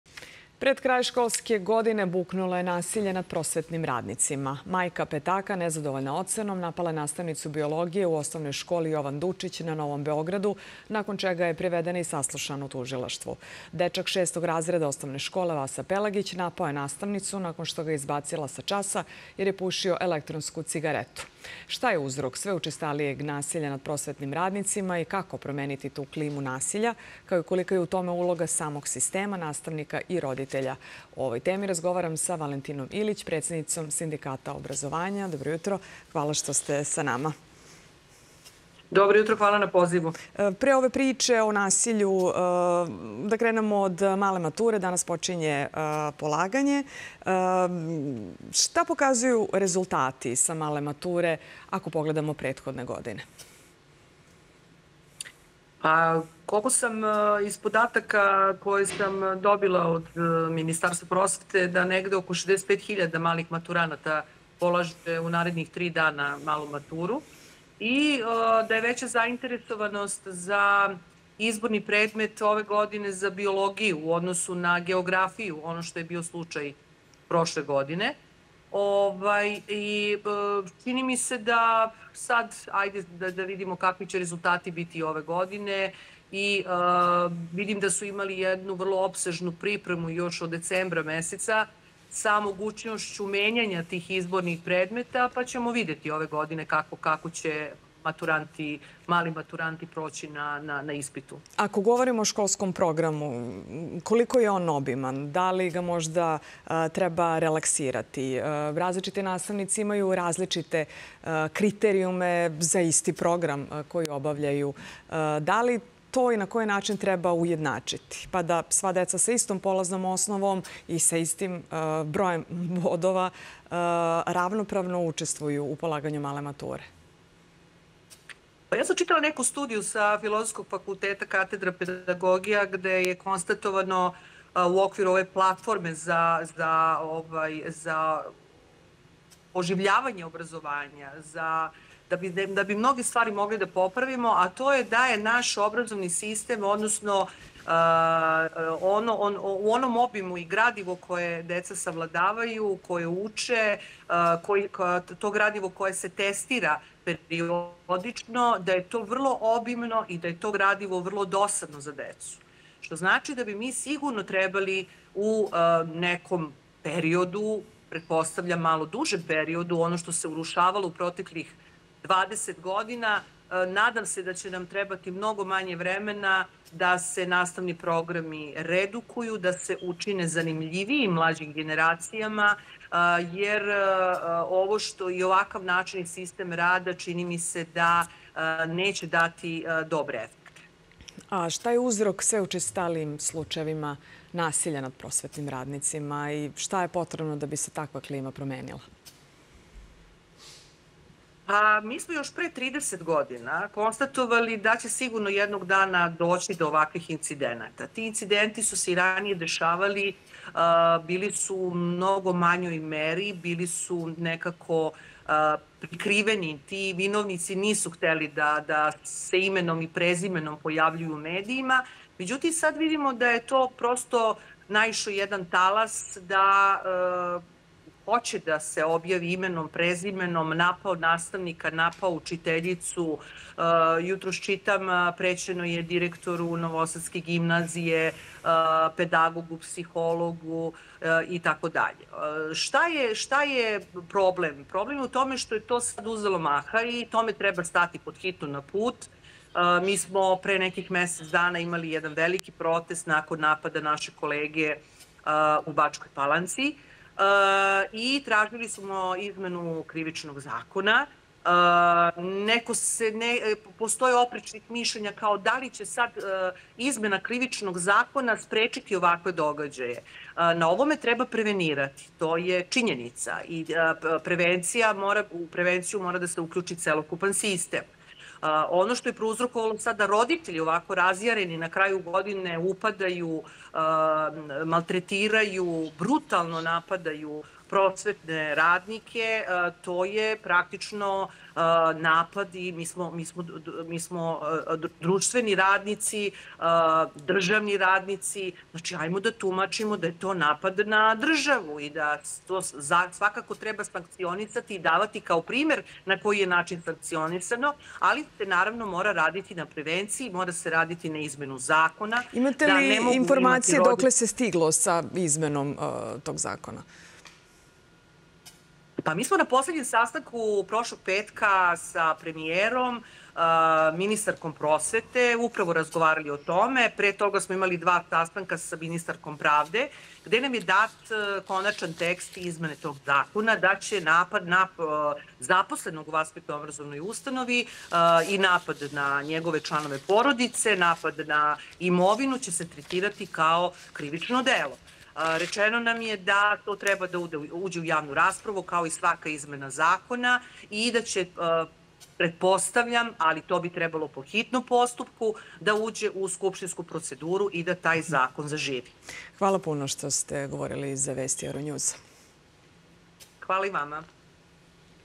Ilić: Neophodne promene krivičnog zakona zbog napada na nastavnike
Predsednica Sindikata obrazovanja Valentina Ilić kaže za Euronews da se slučajevi nasilja moraju prevenirati. Ona je izjavila i da očekuje i oštrije sankcije koje će doneti izmene krivičnog zakona.